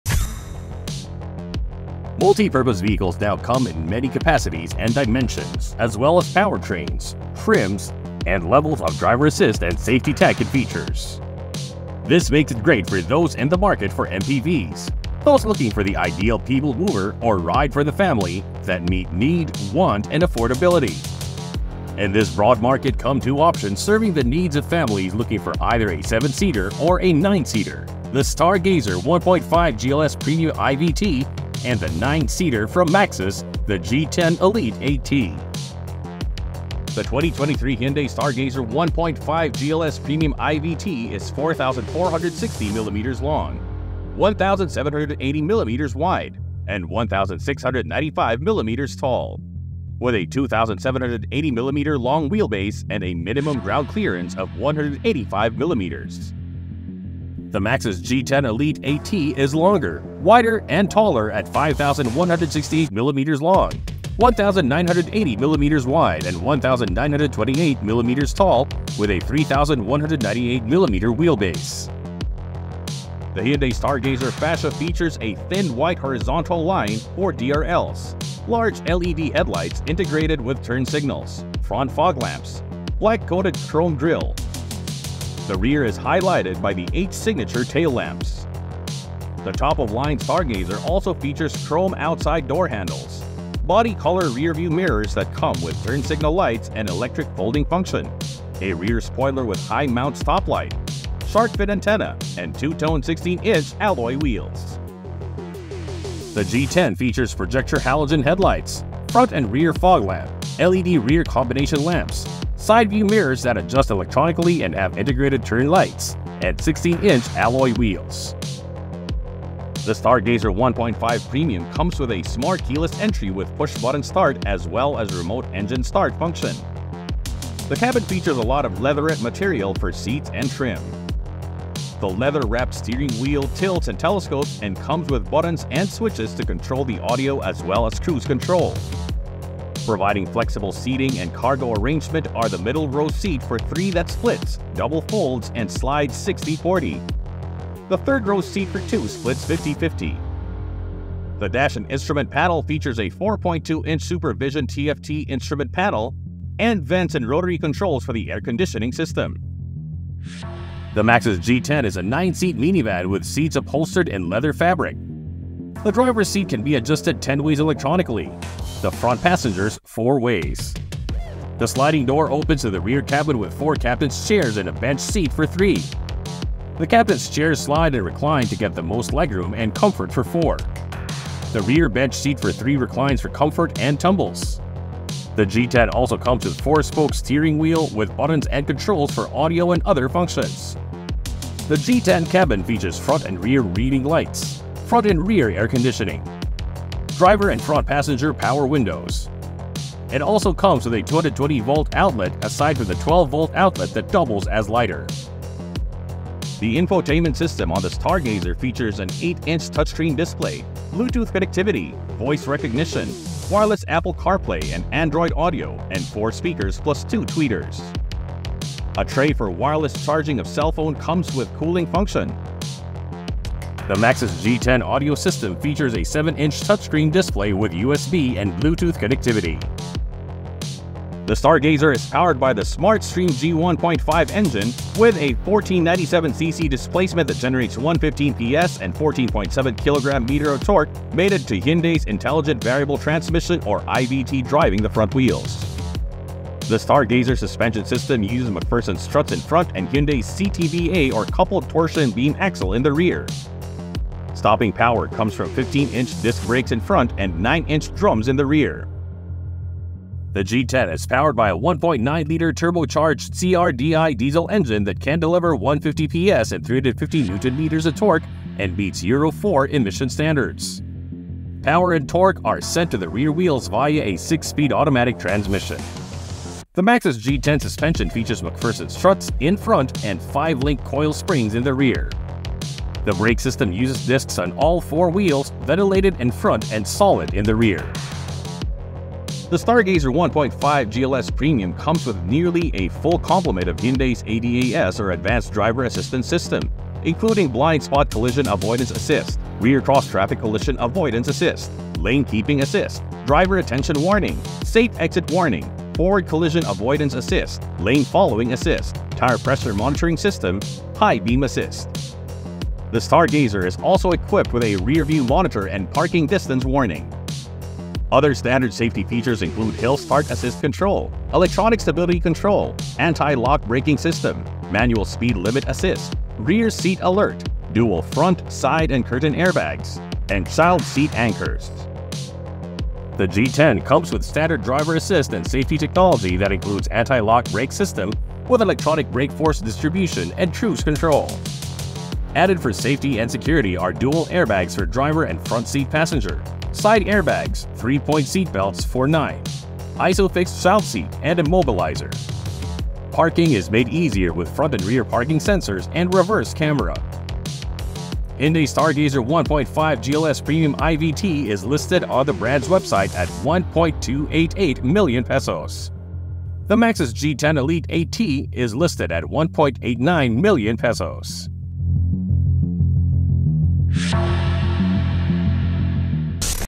Hyundai Stargazer 1.5 GLS Premium IVT vs Maxus G10 Elite AT | Head-to-Head
This edition of Head-to-Head, we check out the specs of two MPVs or multi-purpose vehicles. One is the seven-seater from Hyundai, the Stargazer 1.5 GLS Premium IVT. The other is the nine-seater from Maxus, the G10 Elite AT.